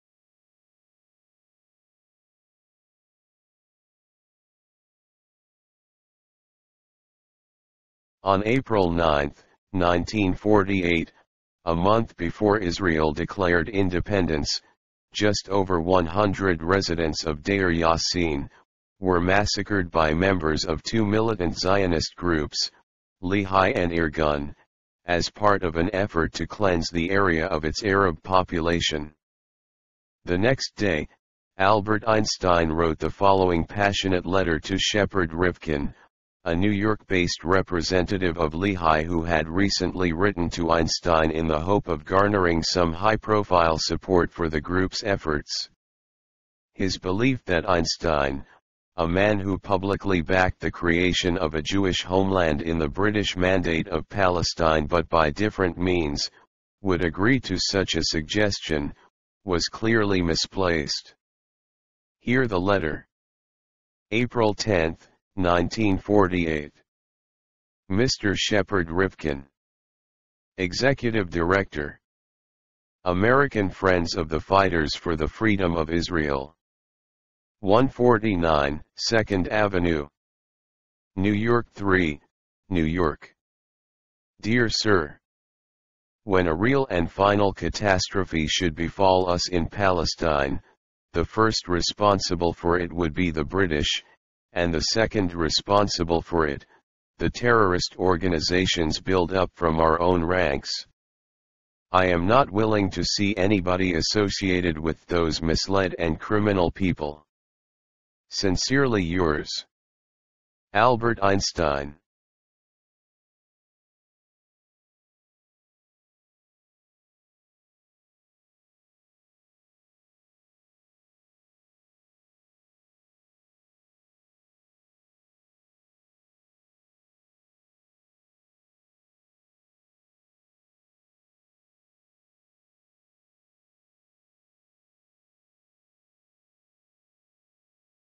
10-4-1948. Einstein letter to Shepard Rifkin the day after the Deir Yassin massacre in Palestine.
On April 9th, 1948, a month before Israel declared independence, just over one hundred residents of Deir Yassin were massacred by members of two militant Zionist groups – Lehi and Irgun – as part of an effort to cleanse the area of its Arab population. The next day, Albert Einstein wrote the following passionate letter to Shepard Rifkin, a New York-based representative of Lehi who had recently written to Einstein in the hope of garnering some high-profile support for the group’s efforts. His belief that Einstein – a man who publicly backed the creation of a Jewish homeland in the British Mandate of Palestine, but by different means – would agree to such a suggestion was clearly misplaced. Here the letter